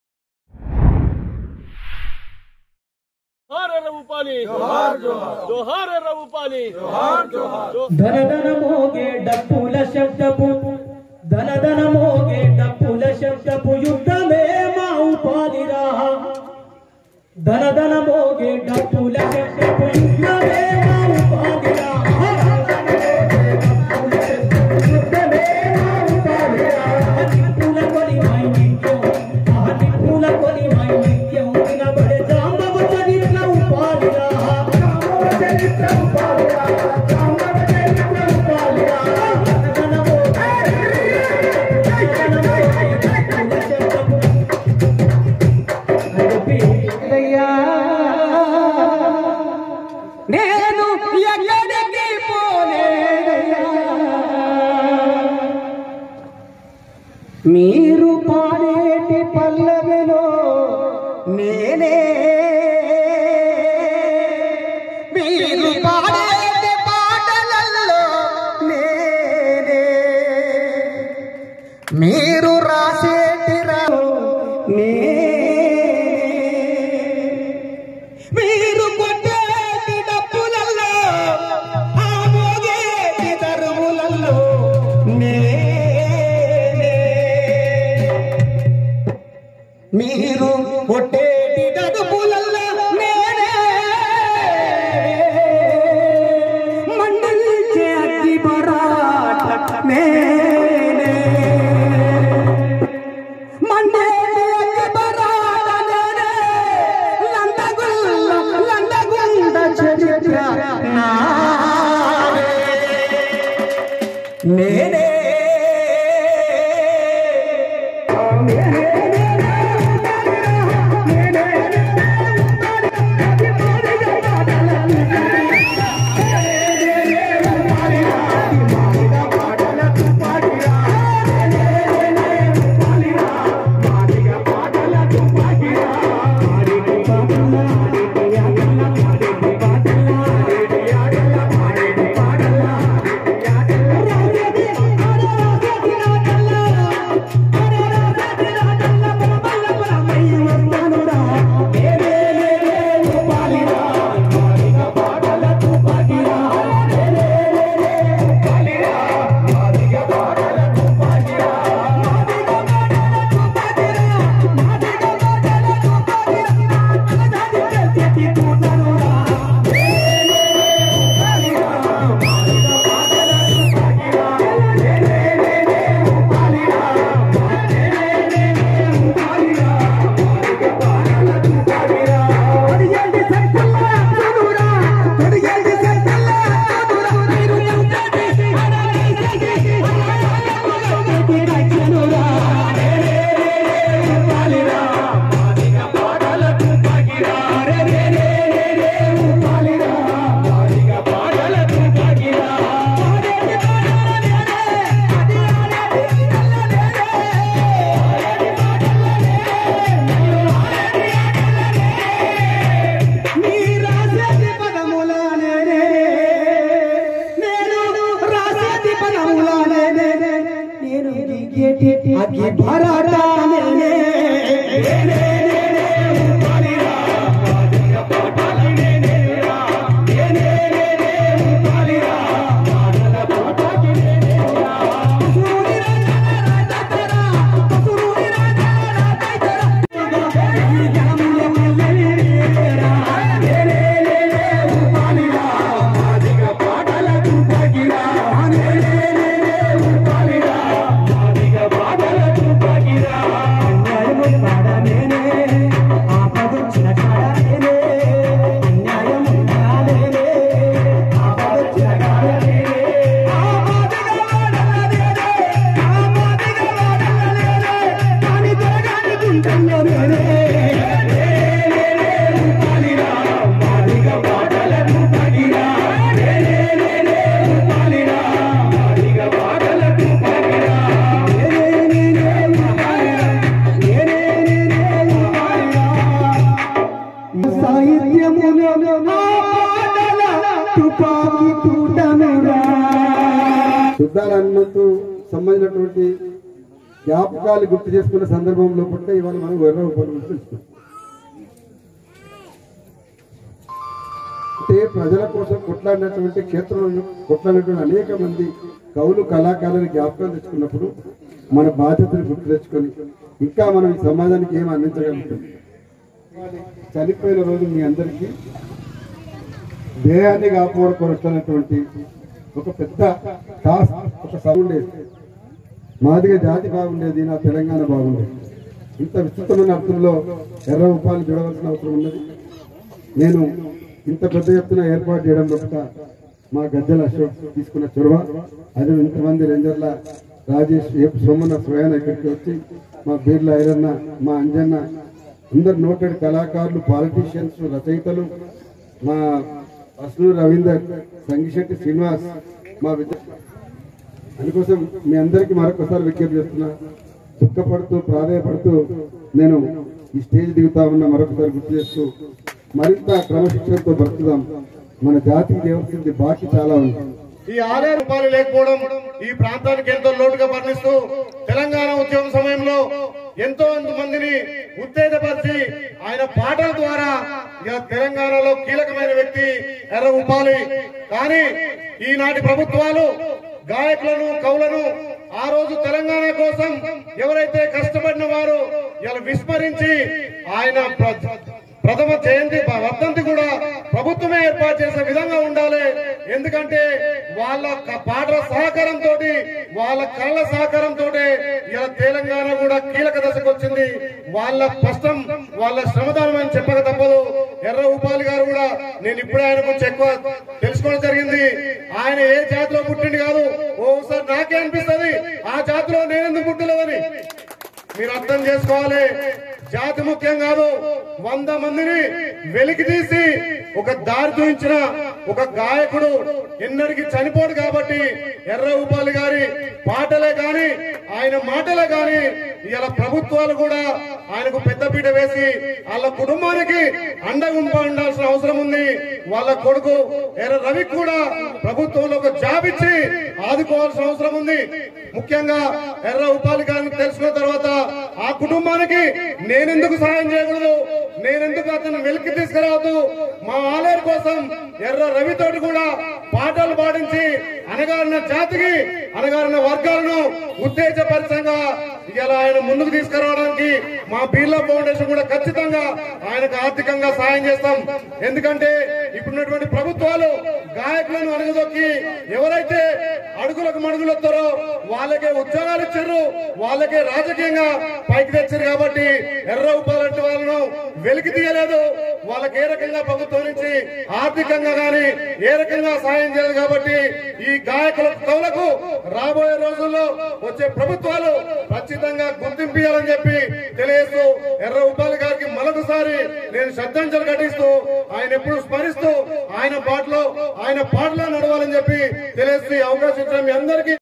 ఎర్ర ఉపాలి ప్రధమ వర్థంతి సందర్భంగా కవులు కళాకారులు అధిక సంఖ్య లో పాల్గొని శ్రద్ధాంజలి ఘటించారు
ఆలేరు న్యూస్